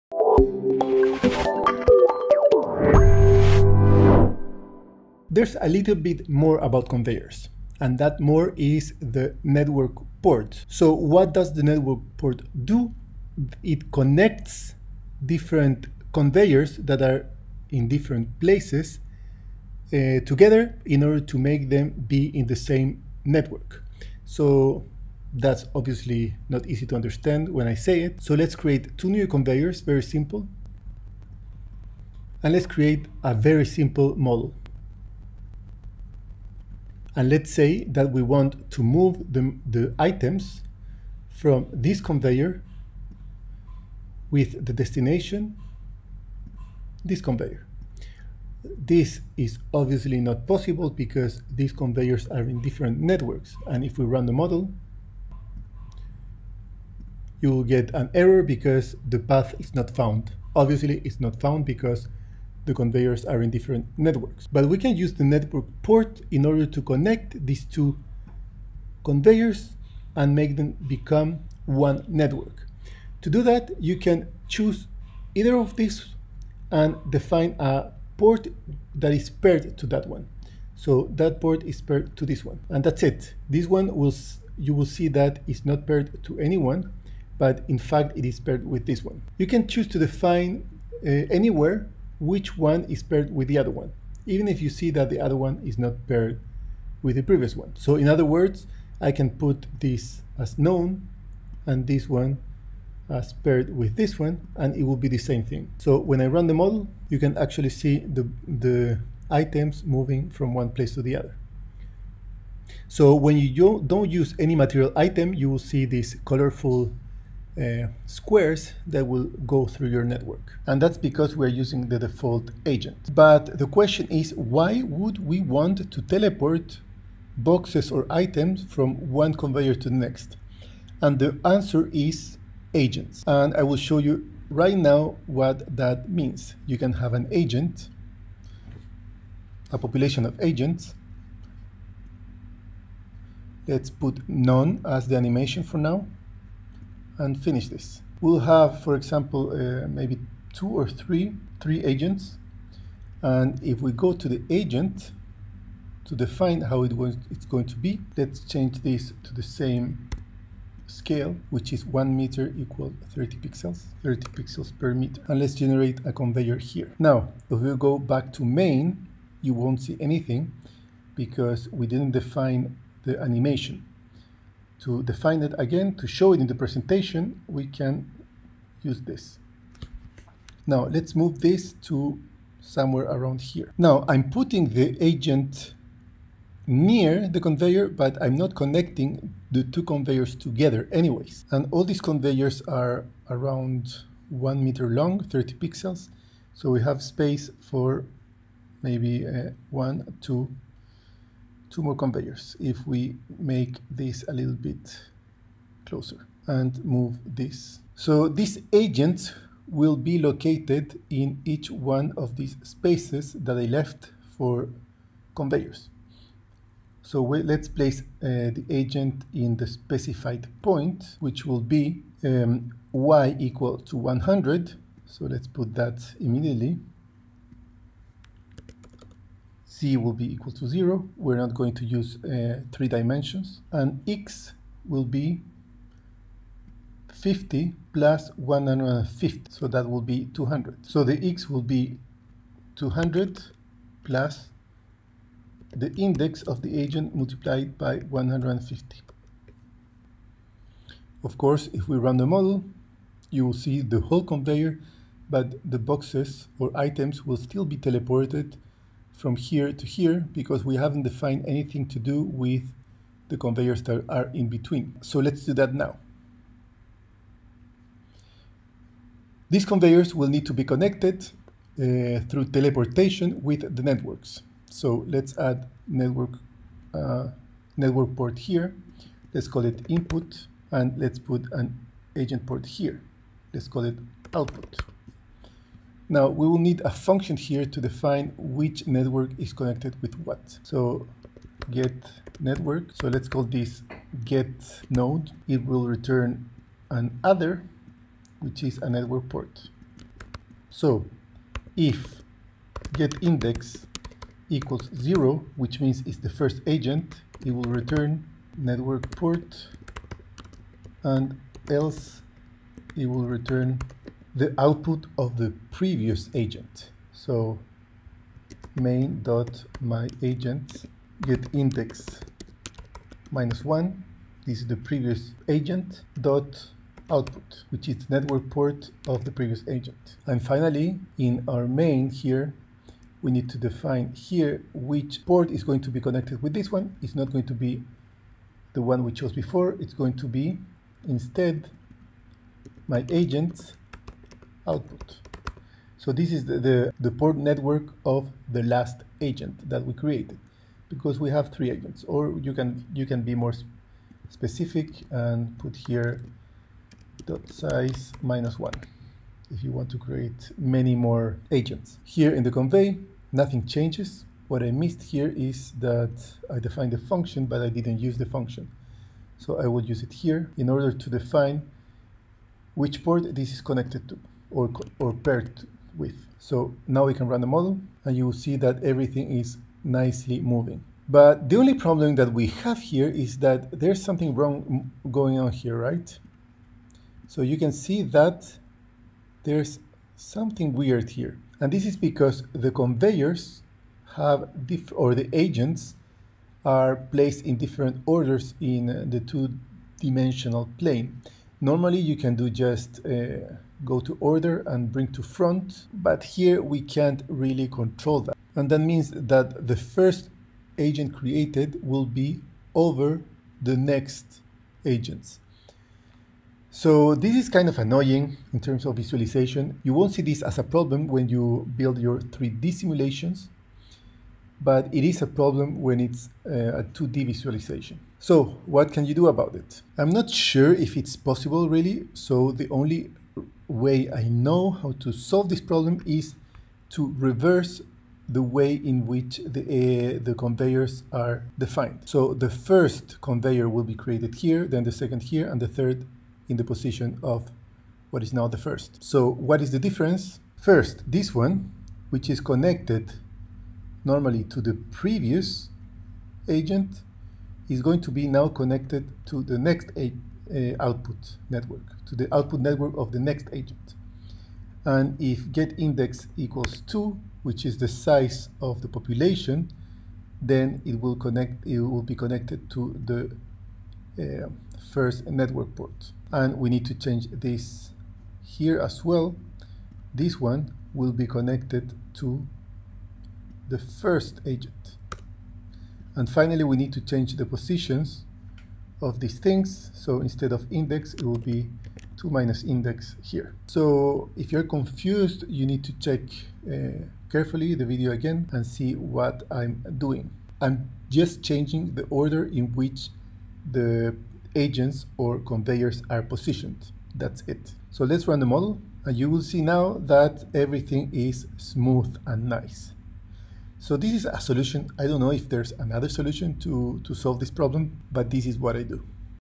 Connecting conveyors present in different agents - Material Handling Library - AnyLogic
Learn how to use network ports.